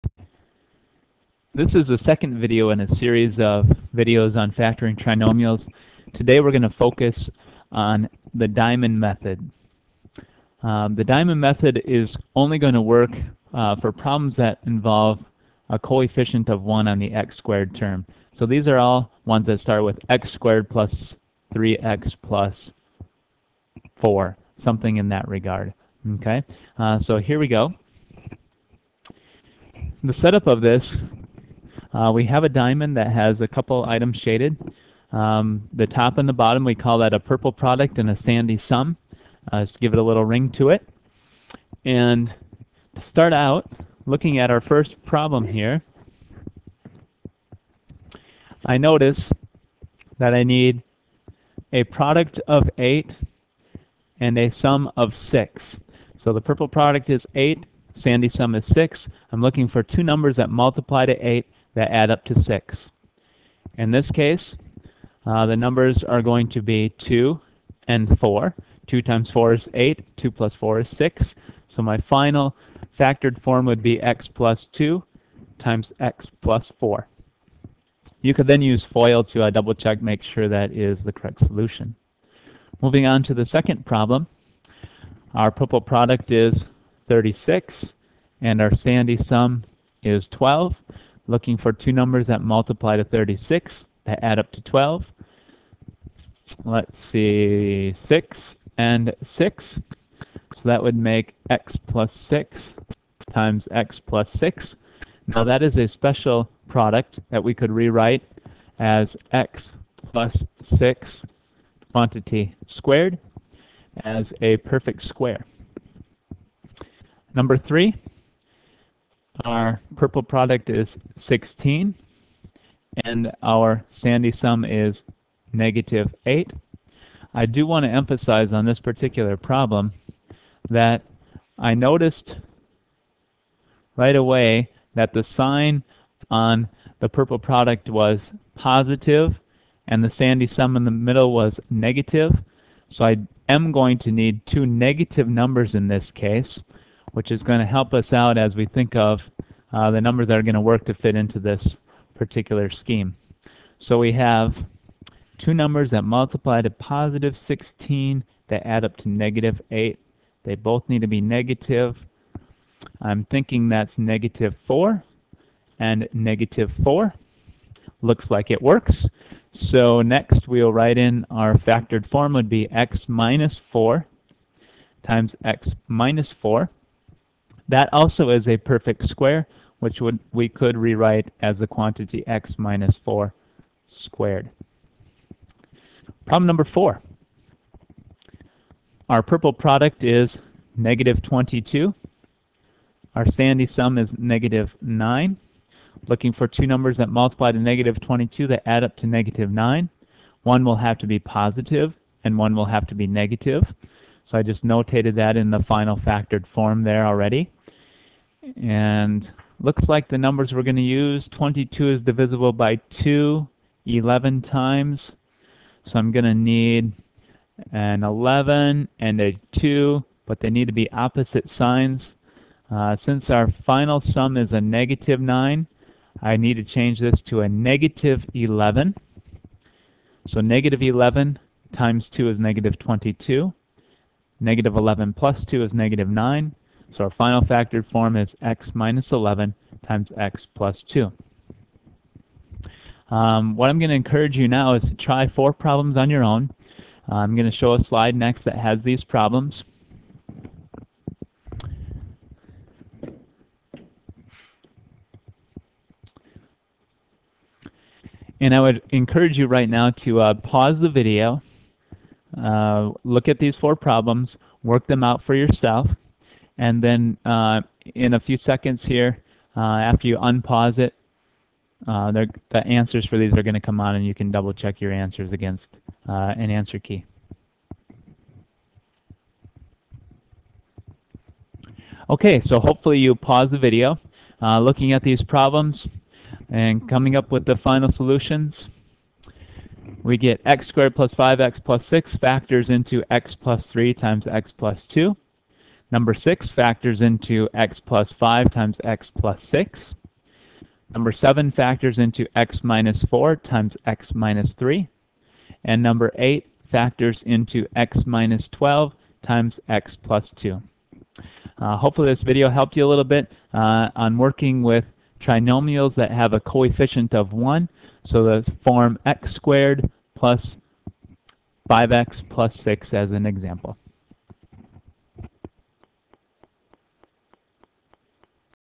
Factoring Trinomials Diamonds for You Tube.wmv
Help with factoring trinomials when the coefficient on the x^2 term is 1.